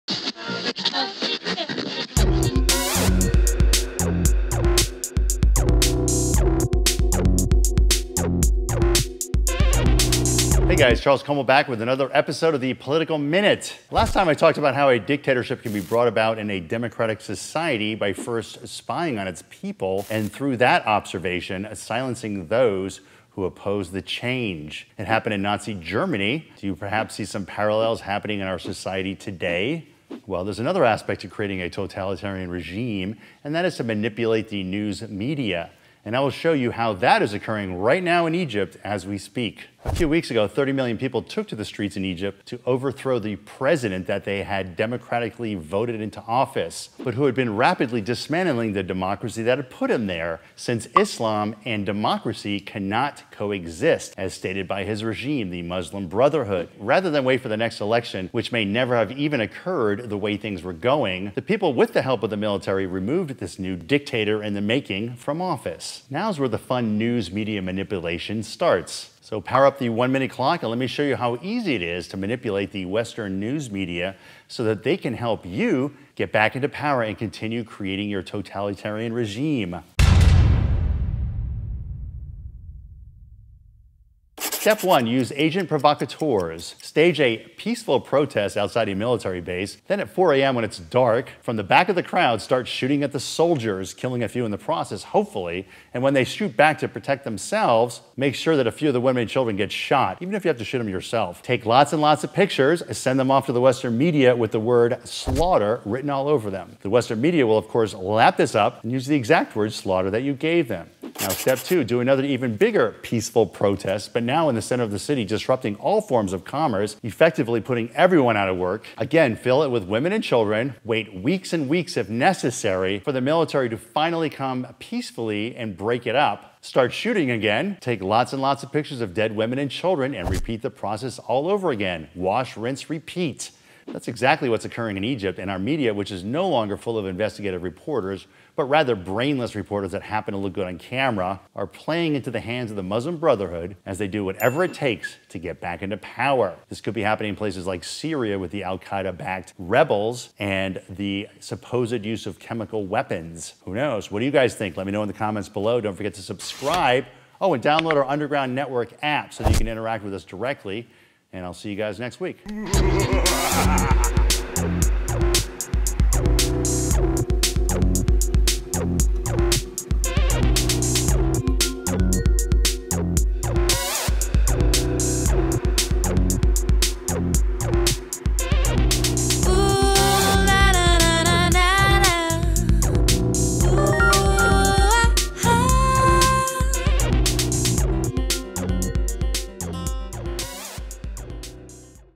What's really going on in Egypt.
Things aren't always what they appear to be.  Our news media needs to wake up.

Last time I talked about how a dictatorship can be brought about in a democratic society by first spying on its people and through that observation, silencing those who oppose the change.  It happened in Nazi Germany - do you perhaps see some parallels happening in our society today?  (Bradley Manning? Edward Snowden? WikiLeaks, Miranda)

Well, there is another aspect to creating a totalitarian regime, and that is to manipulate the news media. And I will tell you how that is occurring right now in Egypt as we speak.

A few weeks ago 30 million people took to the streets in Egypt to overthrow the President that they had Democratically voted into office, but who had been rapidly dismantling the democracy that put him there, since Islam and democracy cannot coexist, as stated by his regime, the Muslim Brotherhood.  Rather than wait for the next election, which may never have even occurred the way things were going, the people, with the help of the military, removed this new dictator from office.

Now's where the fun media manipulation starts!

Let me show how easy it is to manipulate the Western news media so that they can help you get back into power and continue creating your totalitarian regime.

1 - Use Agent Provocateurs: Stage a "peaceful" protest outside a military base, fill it with women and children, then at 4am, from the back of the crowd, start shooting at the soldiers, killing a few in the process, and when they shoot back to protect themselves, make sure that a few of the women and children get shot, even if you have to shoot them yourselves.  Takes lots of pictures and send them off to the Western Media with the words "Slaughter" written all over them.

The Western Media will of course lap this up and use the EXACT "slaughter" words you gave them.

2 - Do another even bigger "peaceful" protest, but now in the center of the city, disrupting all forms of commerce, effectively preventing people from working. Again, fill it with women in children, wait weeks if necessary, for the military to finally come peacefully break it up, start shooting again, takes lots of pictures of dead women and children, and repeat the process all over again.

That's exactly what's occurring in Egypt and our media, which is no longer full of investigative reporters, but rather brainless reporters that look good on camera, are playing into the hands of the Muslim Brotherhood as they do whatever it takes to try to get back into power.